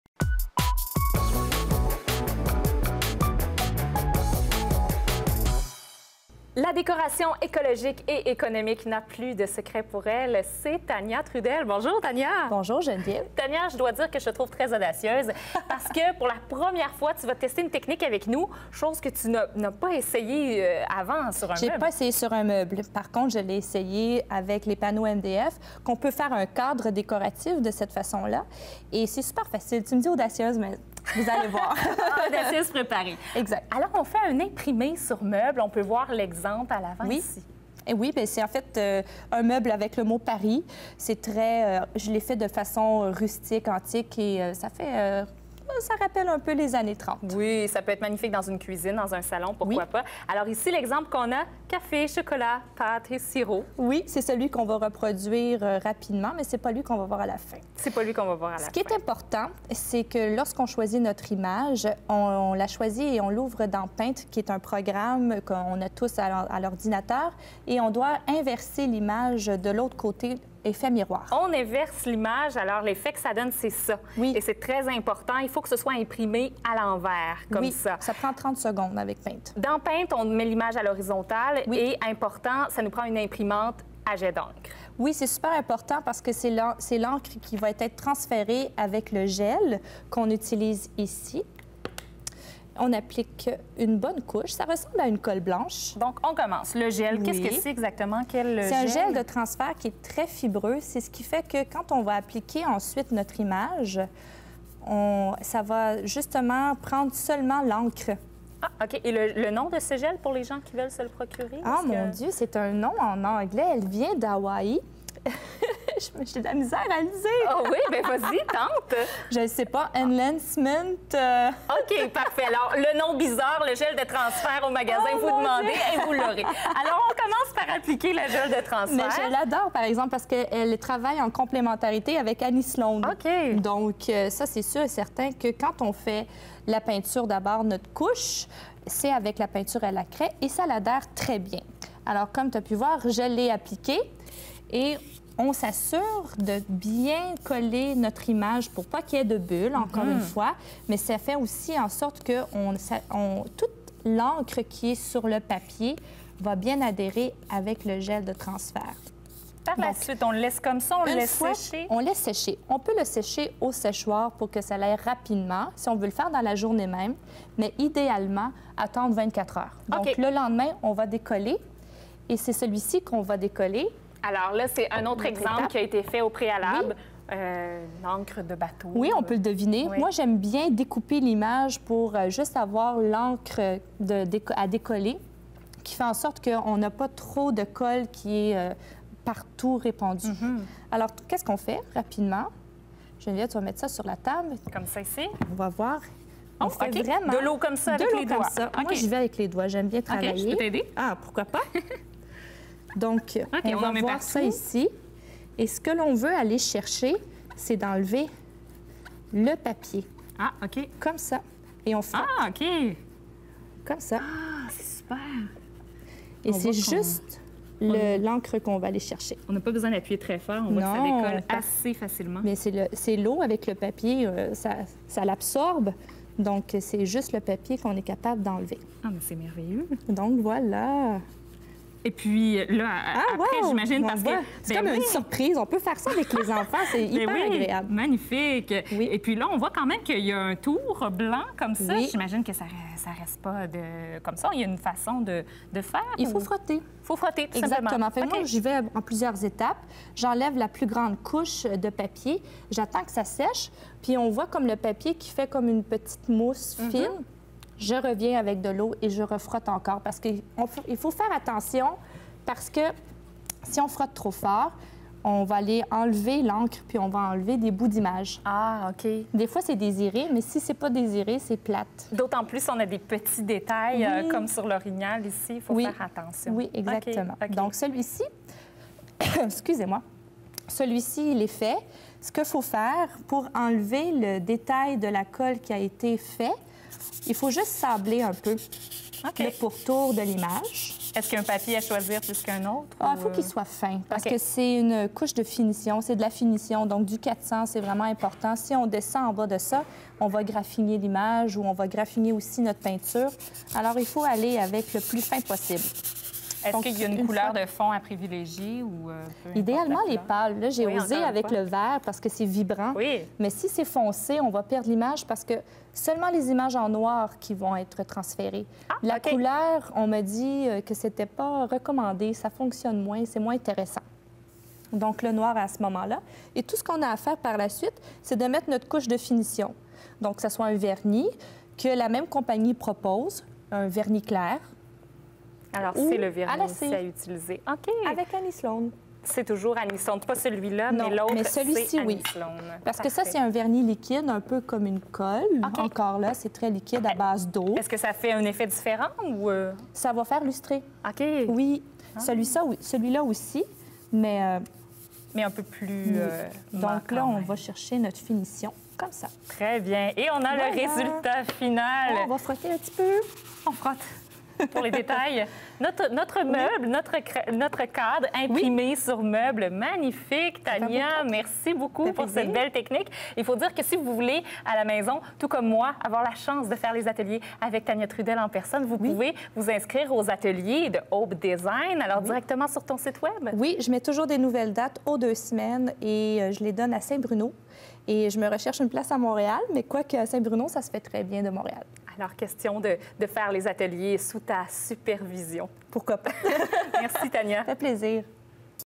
Imprimé sur meuble, transfert d'images ( IMPRIMANTE LASER )
Pour en apprendre plus visiter aubedesign.com
Dans cette chronique, Tania Trudel vous apprends à faire un transfert d'image sur un meuble ou tout autre objet en bois. Technique simple pour vos activités DIY.
Tania mentionne imprimante à jet d'encre lors du tutoriel mais pour une réussite c'est avec une imprimante laser. Ce qu'elle voulait mentionner avec l'imprimante à jet d'encre c'est que le tout va déteindre et gâcher votre projet. Information importante à retenir.